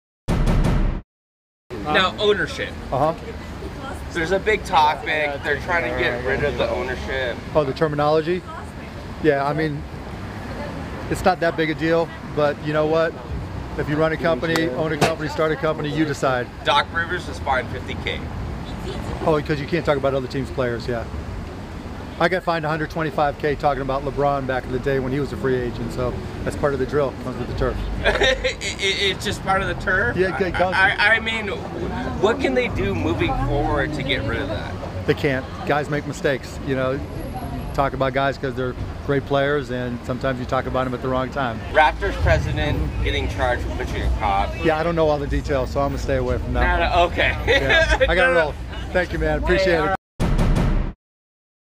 Mark Cuban On NBA 'Owner' Term, 'It's Not That Big a Deal' | TMZ Sports
Mark Cuban ain't losing sleep over the controversy in the NBA over the term "owner" -- telling TMZ Sports, "It's not that big a deal."